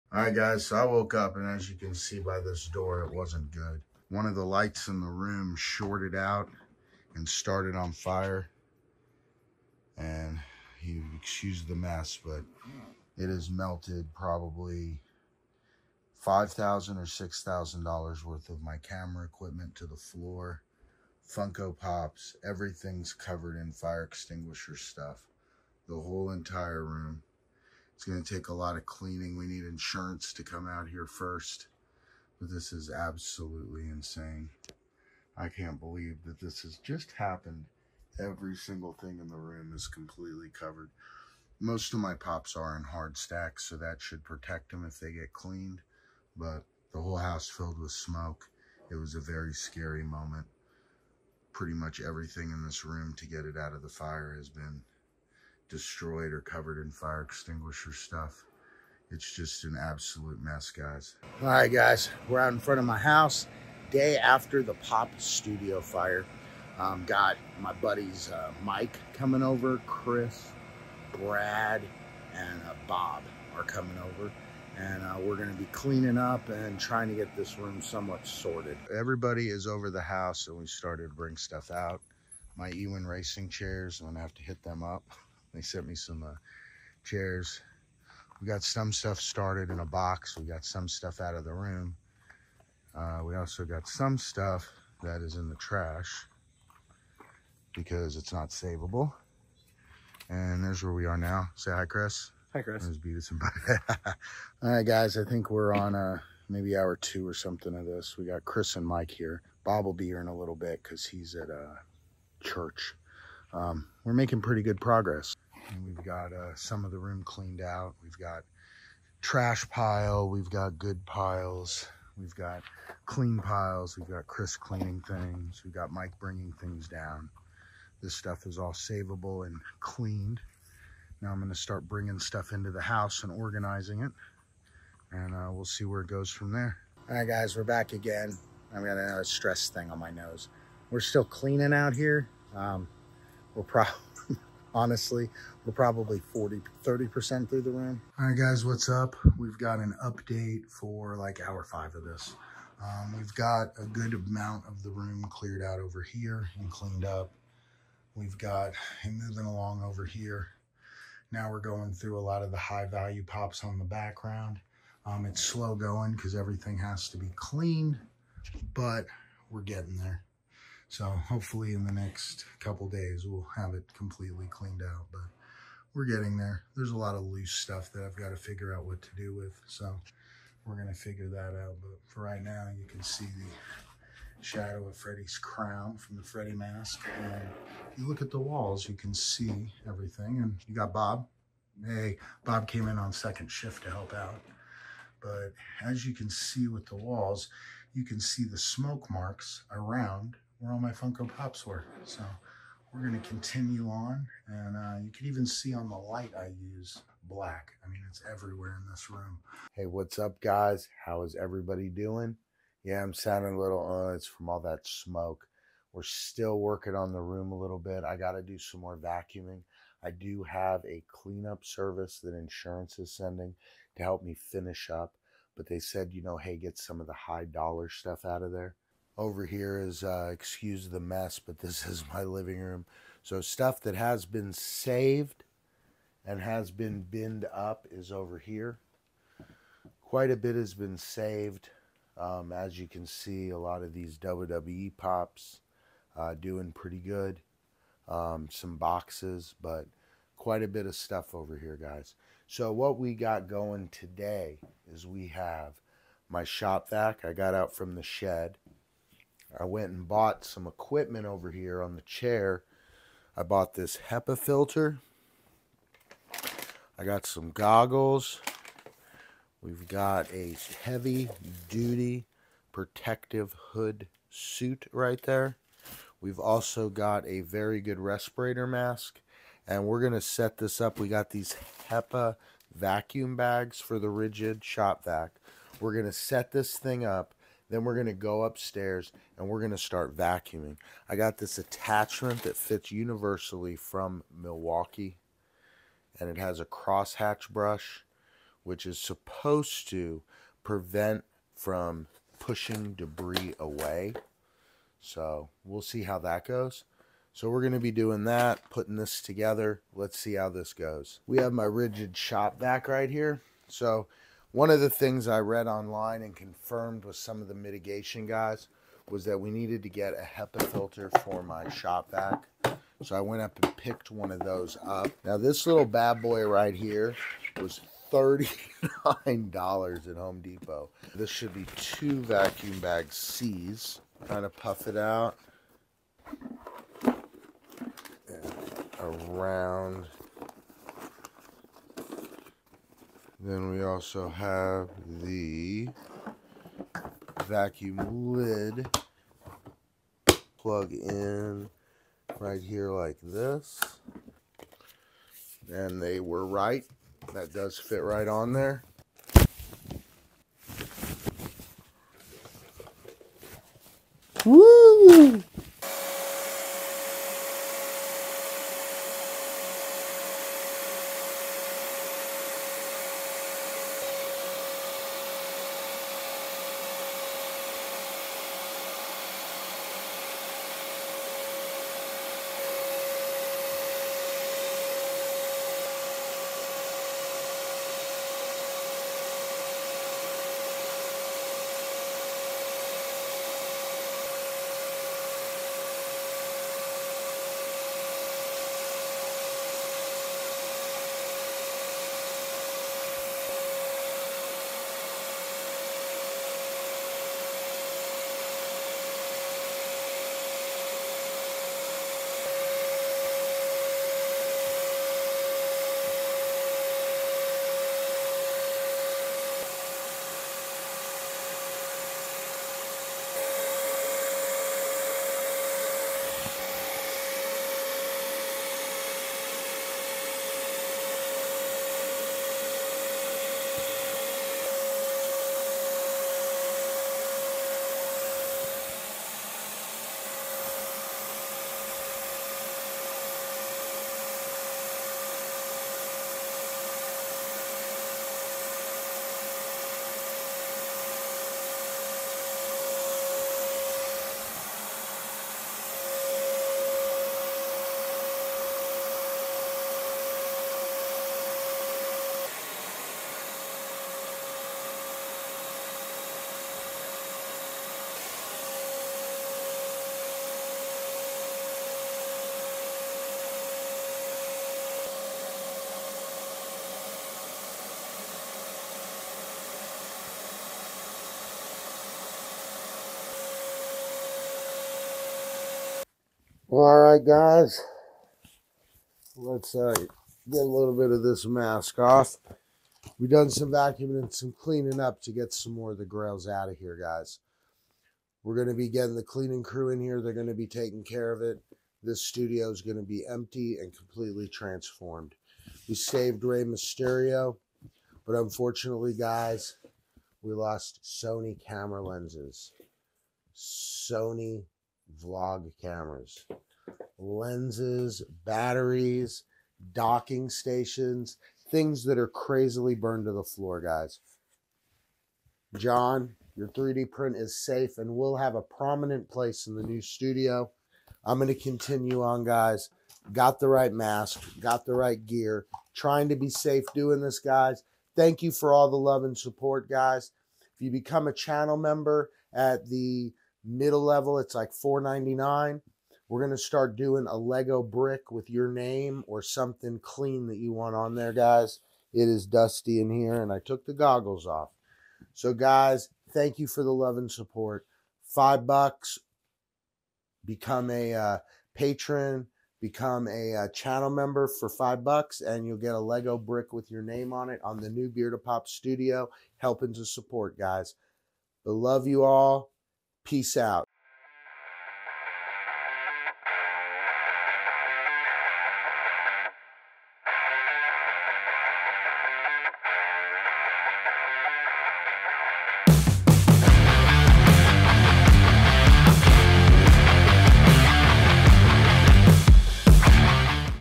Fire in the Funko Pop Studio! How I Saved My Collection!
What started as a typical day in the Funko Pop studio became a nightmare! A fire broke out, and I had to act fast to protect my collection. In this video, I’ll take you through the scary moments, how it happened, and what I did to save my prized Funko Pops. Stick around to see the aftermath and how I plan to rebuild the studio.